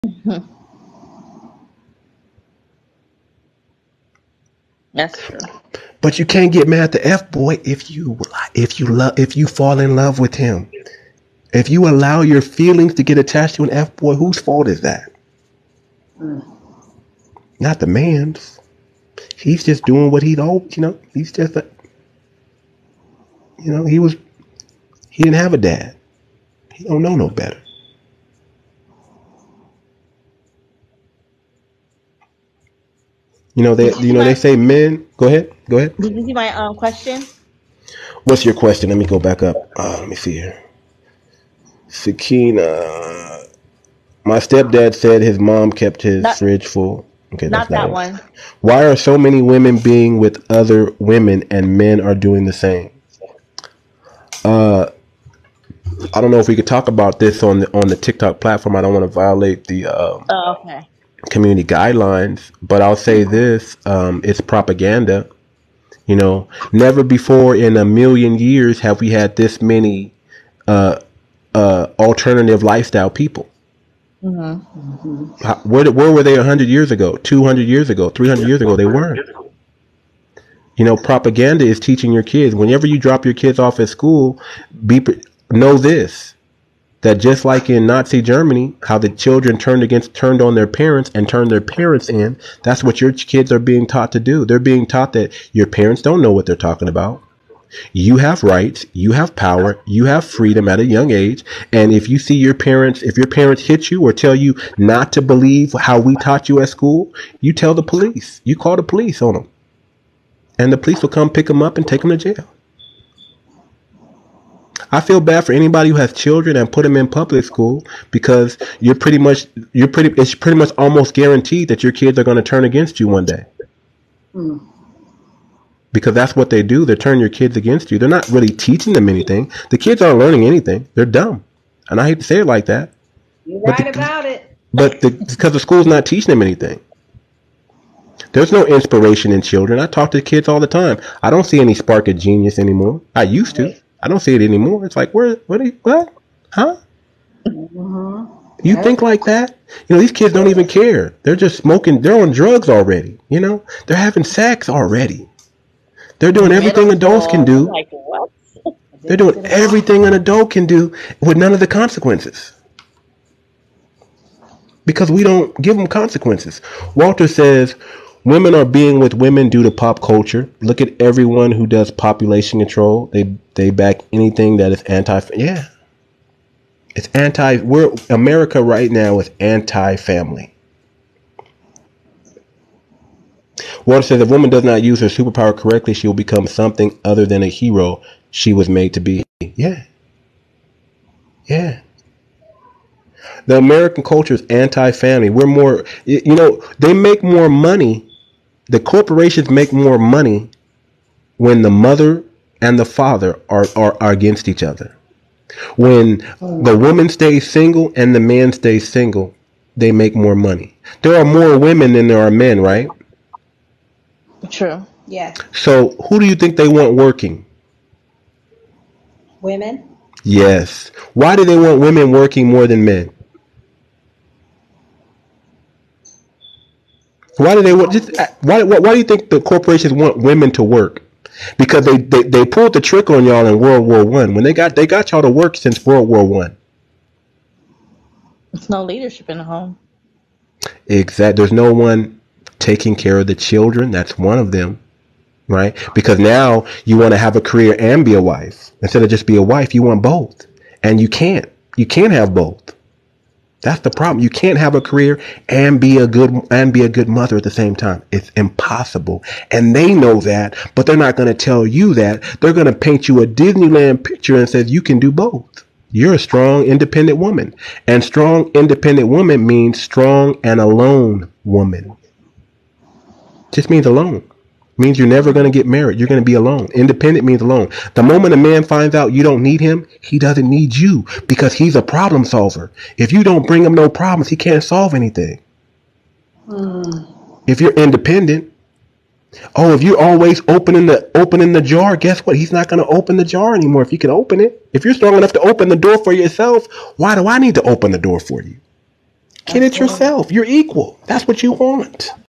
Revealing the Truth: How Dysfunctional Homes Fuel Corporate Profits
Have you ever wondered why so many women are with other women and men are doing the same? In this video, we discuss the propaganda behind this trend and how it's affecting the family dynamic. We talk about how corporations are making more money when the mother and father are against each other and how women are being told they can have a career and be a good mother at the same time, when it's impossible. We also discuss how the moment a man finds out a woman doesn't need him, he doesn't need her.